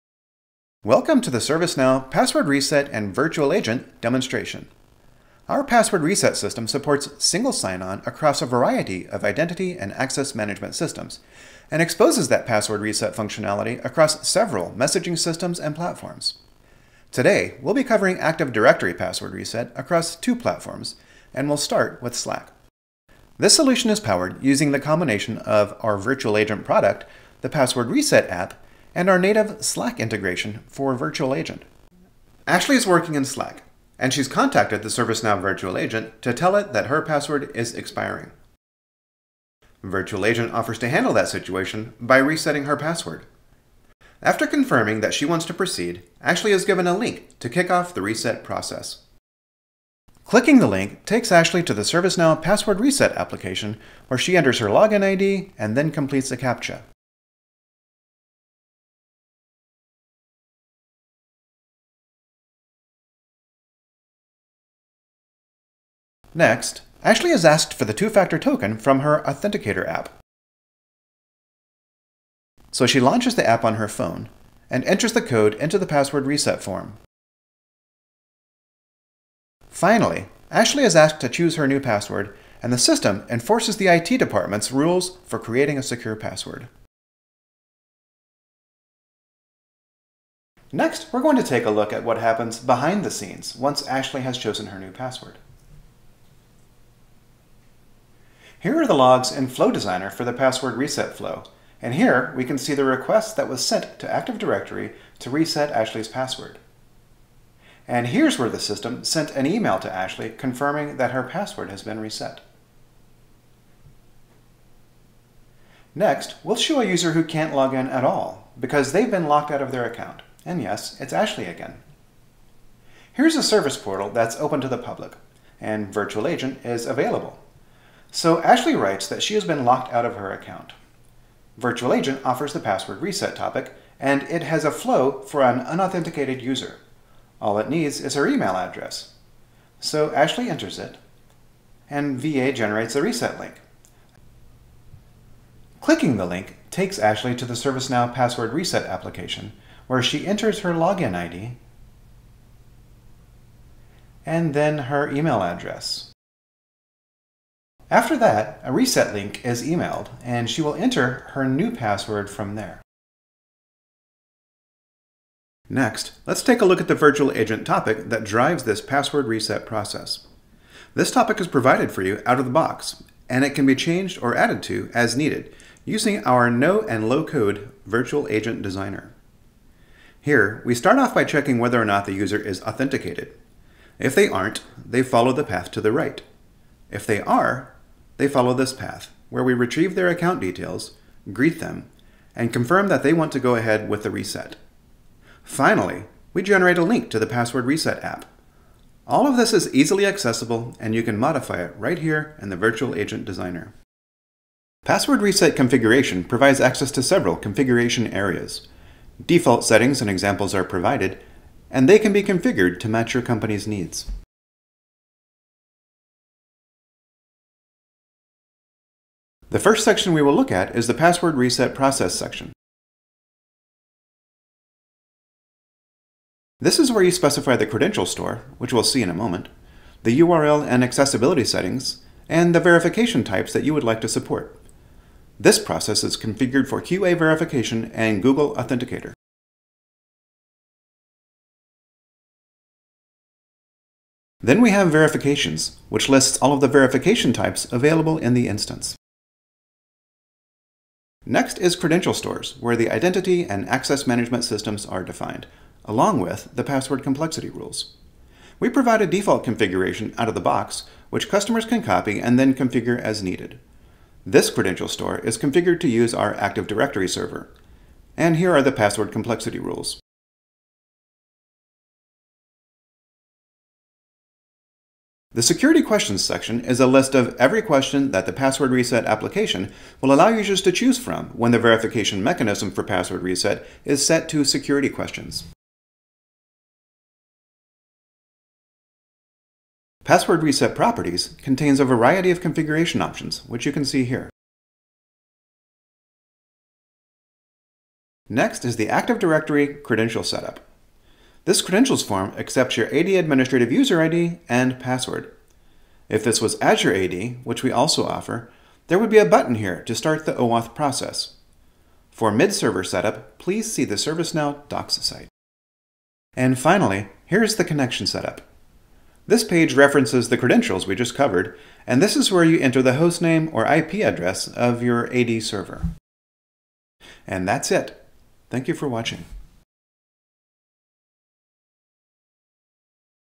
Demo - Password Reset and Virtual Agent
Reduce IT helpdesk ticket volume by up to 30% and increase agent productivity with ServiceNow Password Reset automation for Active Directory and Azure Active Directory. This hands-on demo (Paris release) walks you through (1) the end user experience in Virtual Agent in Slack and Service Portal (2) Active Directory automation powered by Flow Designer and IntegrationhHub (3) Virtual Agent Topic (4) Admin configuration highlights